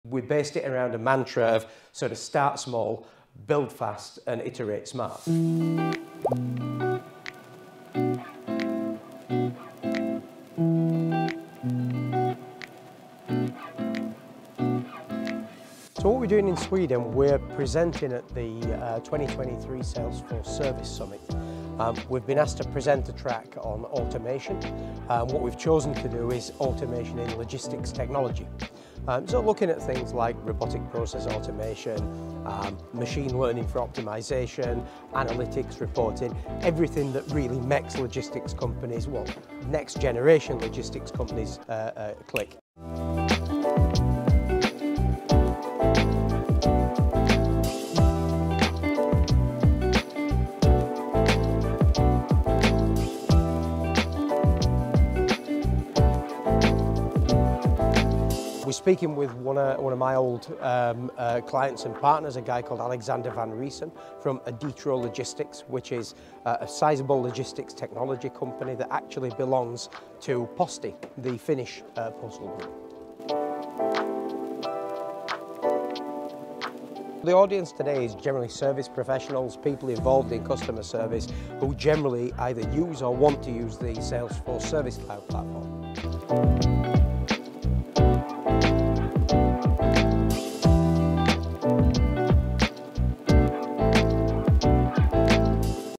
Salesforce: The Heart of Service Summit | Stockholm 2023
TTMS Nordic supported the Salesforce event in two ways:
- as a content partner - Ben Cooper (Sales Manager) gave a lecture on Service Cloud.